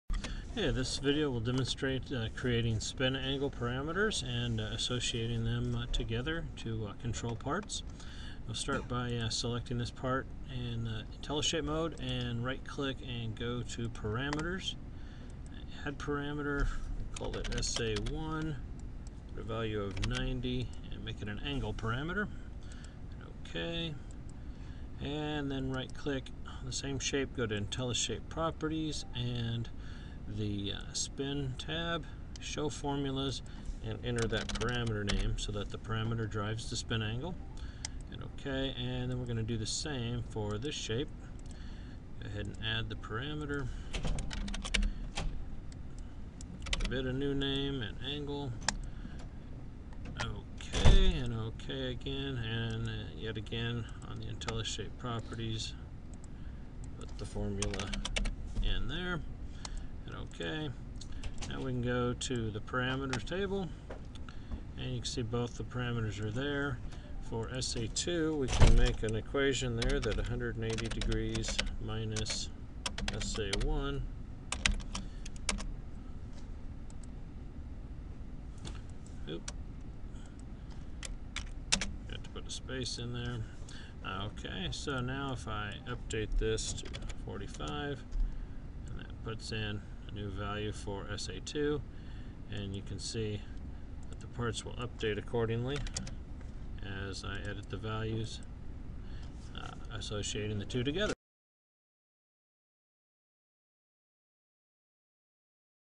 Setting the Spin Angle Parameter in IRONCAD
Example illustrating how to use parameters to control a spin feature's angle in IRONCAD.